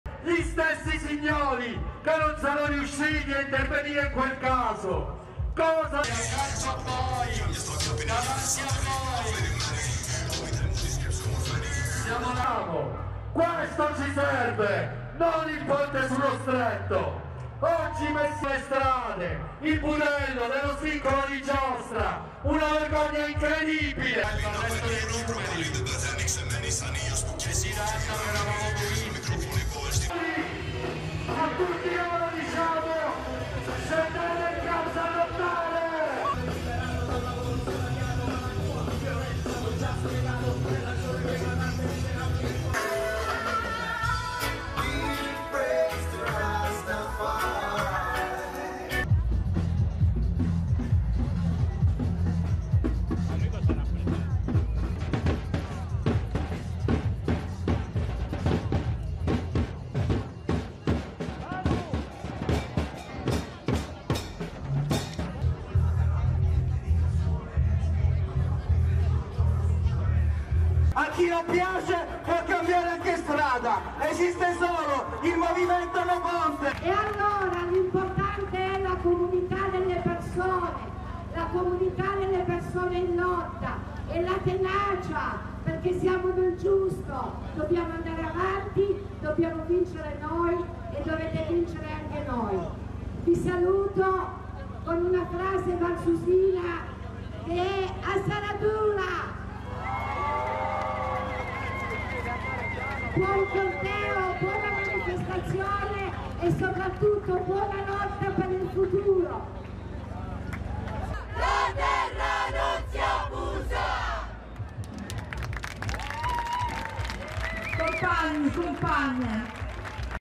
A Messina torna in piazza il comitato No Ponte: "Dobbiamo vincere noi"
Migliaia di persone a Messina alla manifestazione contro la realizzazione del ponte sullo Stretto. Il popolo del No ponte è tornato in piazza, questa volta con un corteo partito da piazza Cairoli in pieno centro città. Migliaia di persone arrivate da tutta la Sicilia e anche dalla Calabria manifestano con striscioni, bandiere, cartelli , slogan contro un'opera considerata inutile e costosa e per chiedere strade, autostrade e ferrovie e tutte le infrastrutture che mancano in Sicilia e Calabria.